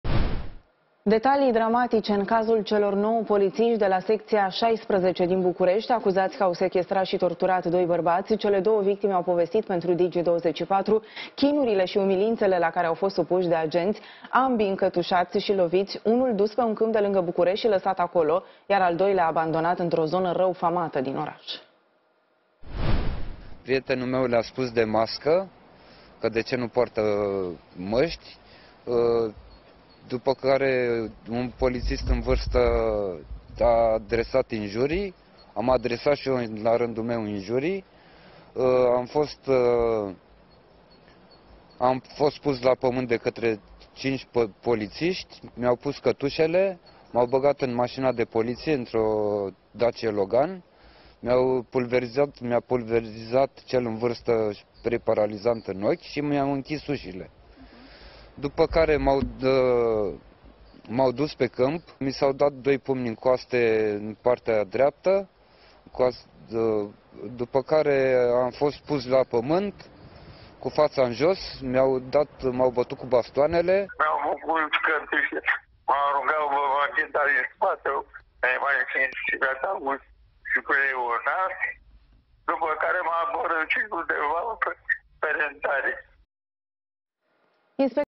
Victimele polițiștilor povestesc torturile: „M-au dus pe câmp. M-au bătut cu bastoanele” - Digi24
Detalii dramatice în cazul celor nouă polițiști de la Secția 16 din București, acuzați că au sechestrat și torturat doi bărbați. Cele două victime au povestit, pentru Digi24, chinurile și umilințele la care au fost supuși de agenți.